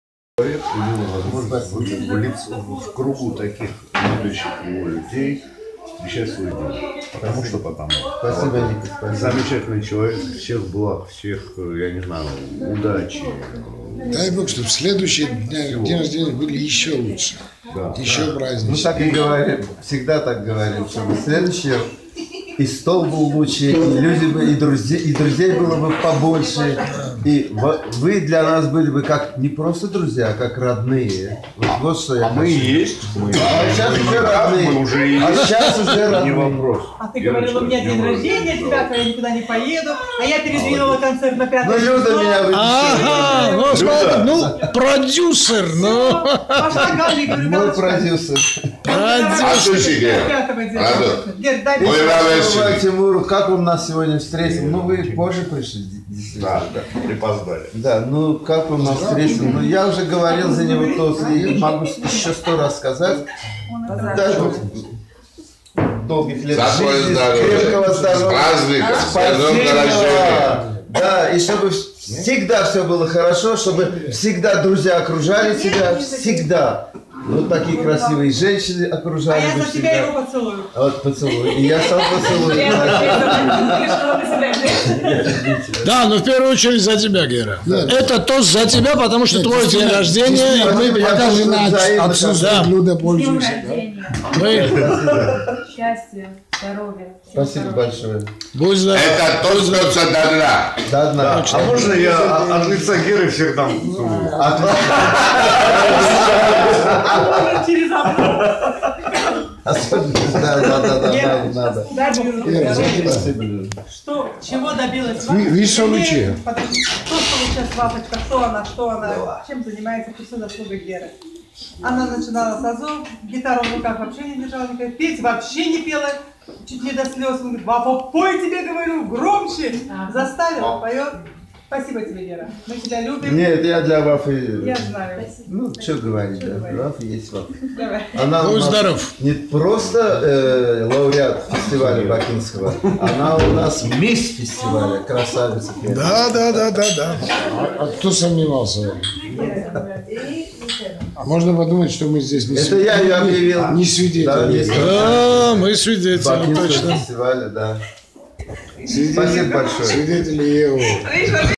2016 год 9 мая. Тбилиси. День рождение нашего бакинского друга Геры Черногорова
В гостеприимной семье Темура и Лии Пагава вечер после одного из концертов международного фестиваля авторской песни плавно перевалил за полночь, в 9 мая, то бишь в день рождения Георгия Черногорова.